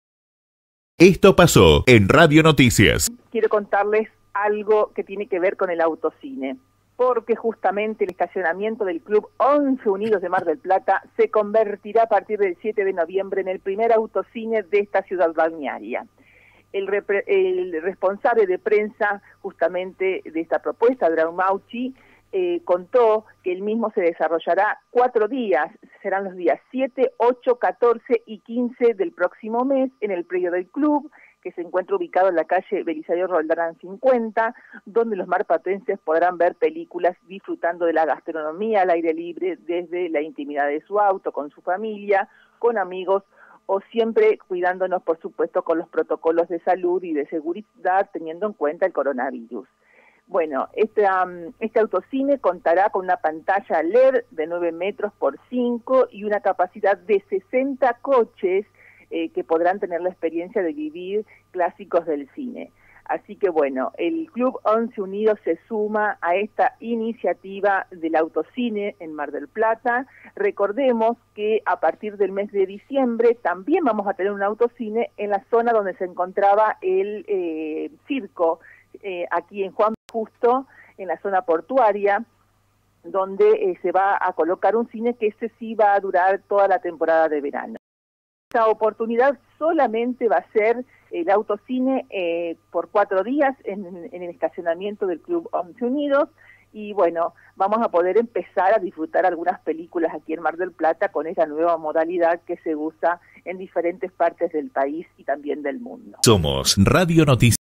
Mar del Plata tendrá su autocine
A raíz de la pandemia por el coronavirus esta temporada se impondrán los autoeventos, y en este marco en noviembre el público podrá probar la primera experiencia de autocine en Mar del Plata. El primer evento de estas características se podrá disfrutar durante 7, 8, 14 y 15 de noviembre en el estacionamiento del Club Once Unidos.

Así, el club ubicado en Belisario Roldán 52 será el marco para ver una película y probar la modalidad de autocine en Mar del Plata. A su vez, la propuesta invita a “disfrutar de la oferta gastronómica al aire libre desde la intimidad del auto con el grupo familiar o amistades pero cumpliendo con los protocolos de salud, seguridad e higiene” que hubo que adoptar dada la situación epidemiológica.

El primer autocine en Mar del Plata contará con una pantalla de led de 9 x 5 metros y una capacidad de 60 para autos. El público podrá disfrutar durante cuatro días de varias películas con sonido que será transmitido por FM en los vehículos.

Esta experiencia tendrá lugar los dos primeros fines de semana de noviembre. En detalle, será el sábado 7 y domingo 8, y se repetirá el sábado 14 y domingo 15, con funciones a las 19:30 y a las 22:30.

Será la primera experiencia de autocine en Mar del Plata después de la iniciativa que se realizó en el Museo MAR para una edición pasada del Festival Internacional de Cine. Además será la forma de ir regresando a eventos colectivos desde que se comenzaron a implementarse las medidas de aislamiento social preventivo y obligatorio en la ciudad.

Las entradas, por vehículo, tienen un valor a partir de $1500 y ya están a la venta a través del portal articket.com.ar.  Cabe recordar que sólo se van a vender en formato digital y serán validadas al llegar con un escaneo del código QR. Por lo cual, no se venderán entradas en puerta, sólo por la página web.